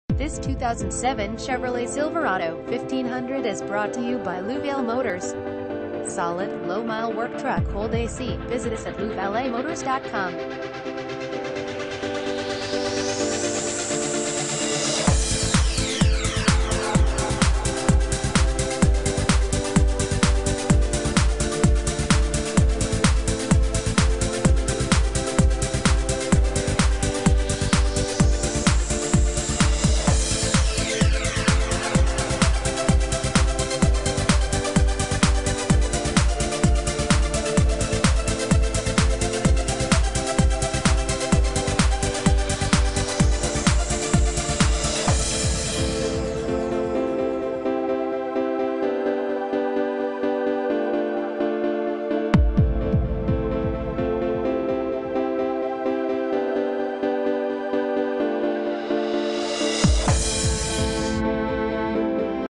2007 Chevrolet Silverado 1500 2WD Reg Cab 8FT BED Work Truck (Houston, Texas)
Louvale Motors
4710 North Shepherd Dr
Houston